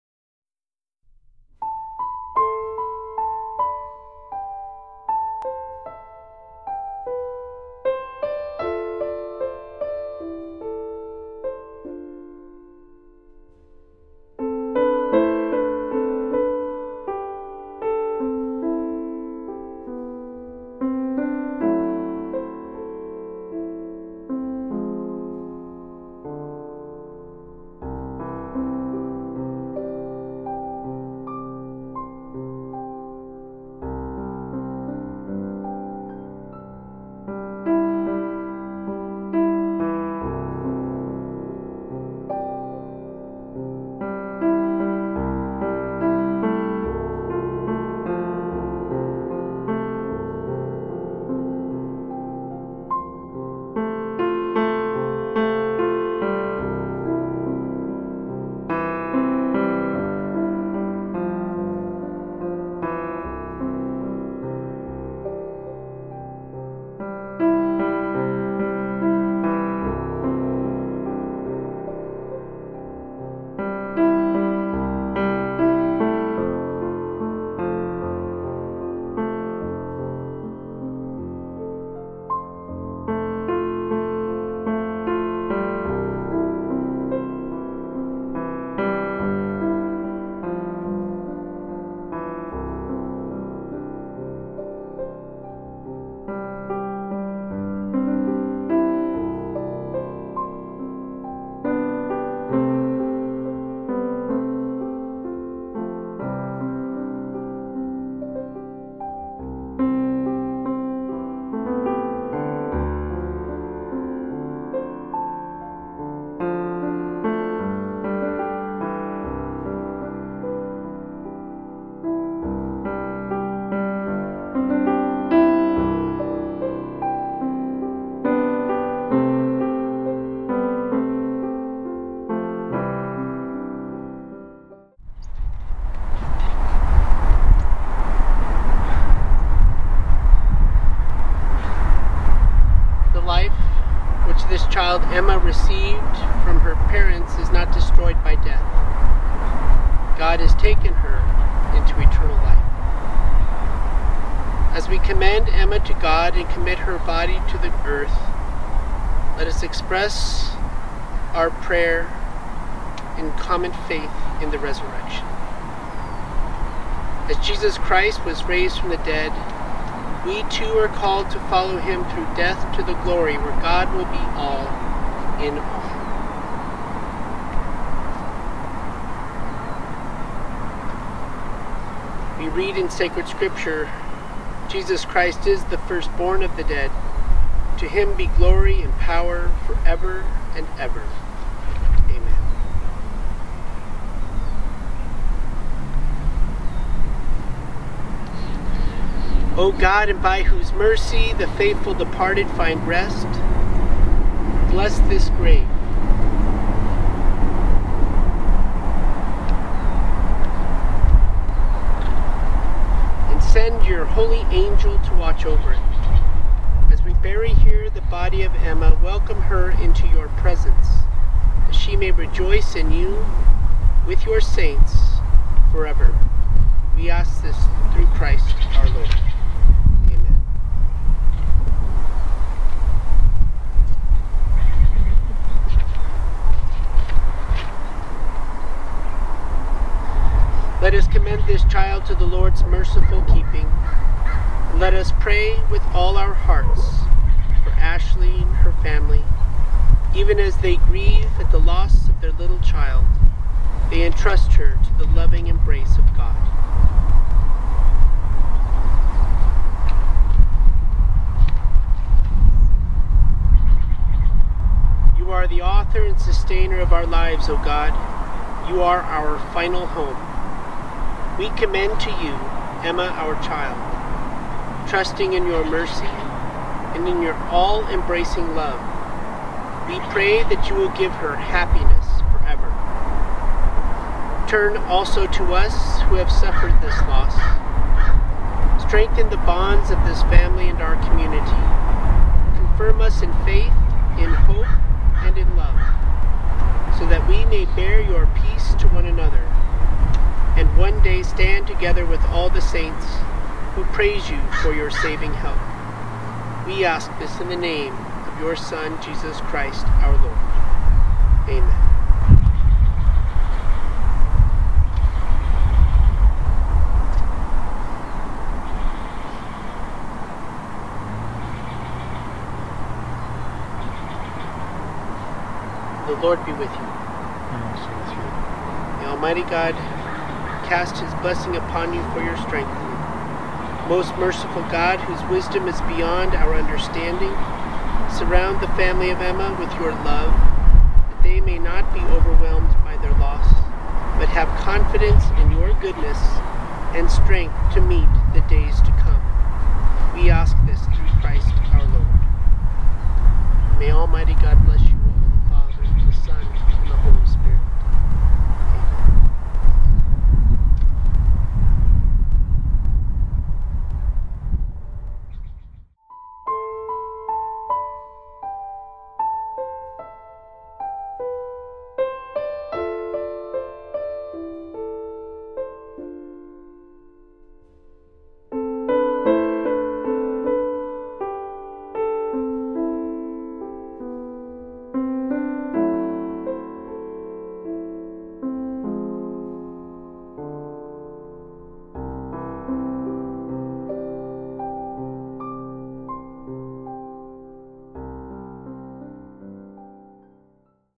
Emma Adiliea Jurado
Memorial service for Emma Jurado
Produced for Litwiller - Simonsen Funeral Home